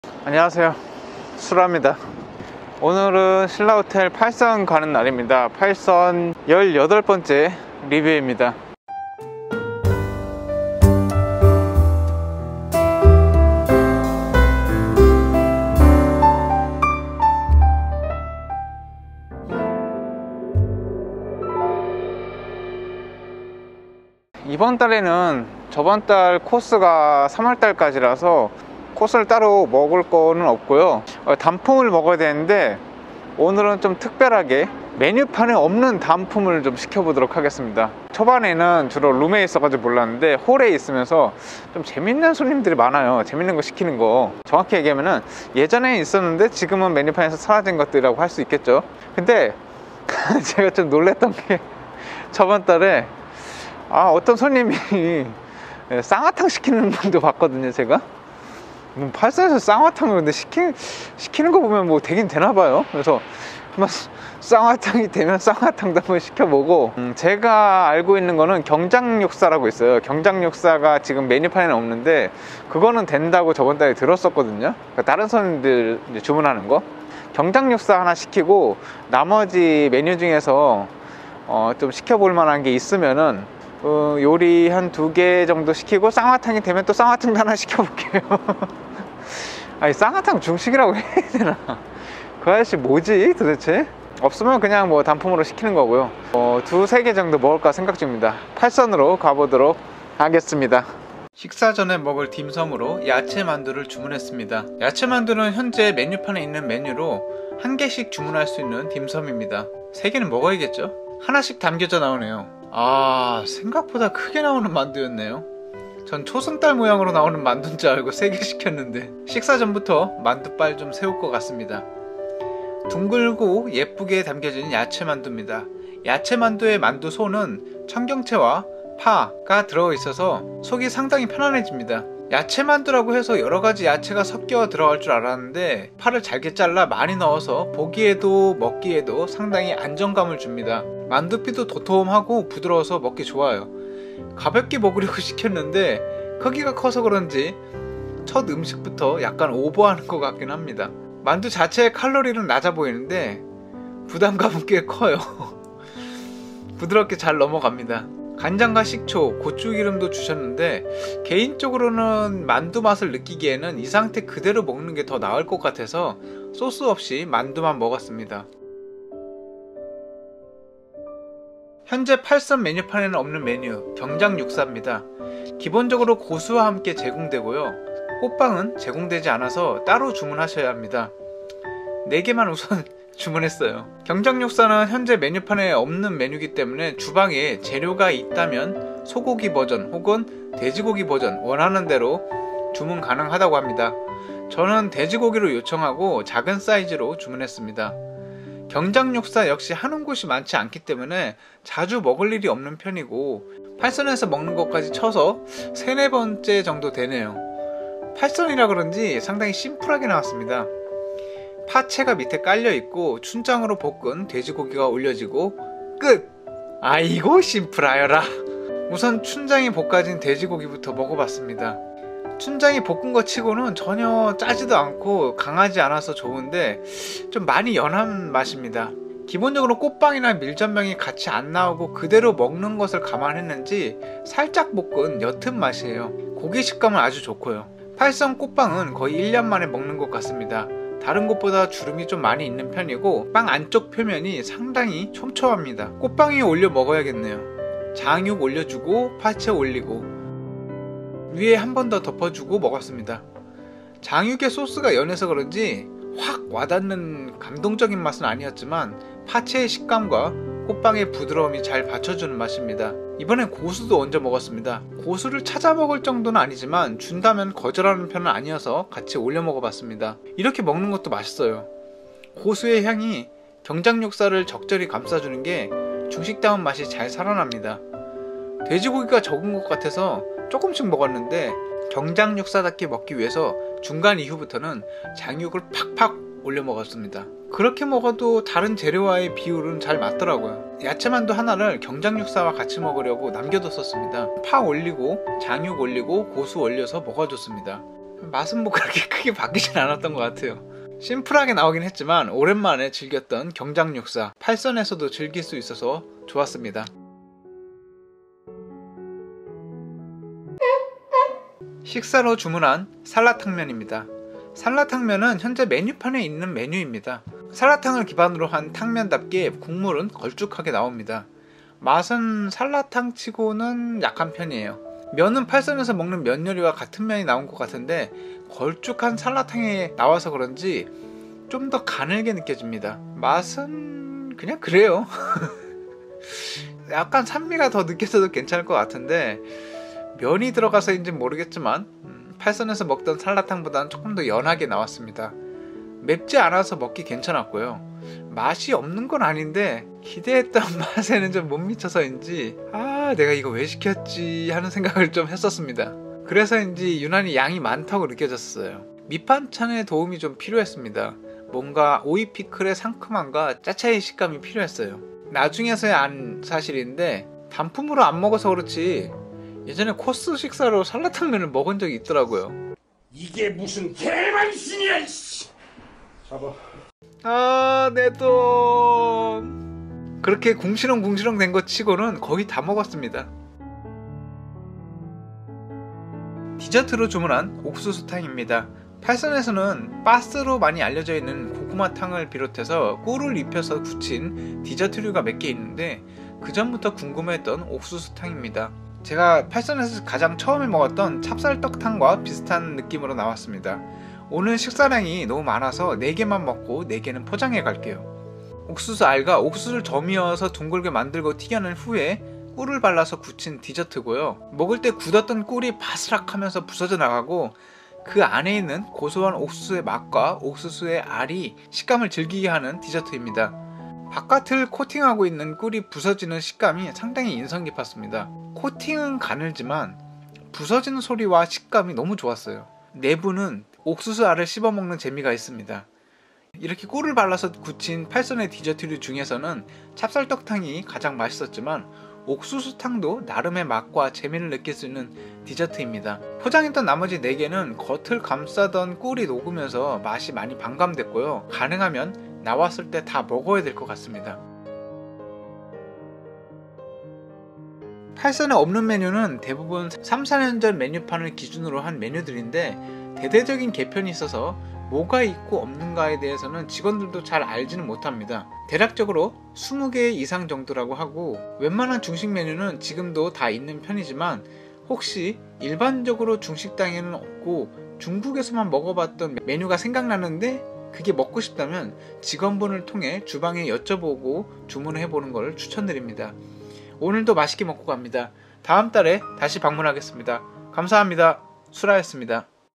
신라호텔 팔선 18th - 팔선에서 안 파는 메뉴를 주문해봤습니다 ㅋㅋㅋ
신라호텔 중식당 팔선 18번째 리뷰입니다

3월 중순에 찍은 건데 3월말에 너무 바빠서 이제서야 올립니다

단품으로 구성해서 먹은 메뉴이고, 팔선에는 없는 메뉴를 주문해봤습니다

하나는 되고, 하나는 안 됐습니다 ㅋ

자세한 사항은 영상을 통해 확인해주세요

[양해바랍니다]
채소만두를 영상에서 자꾸 야채만두라고 하고 있습니다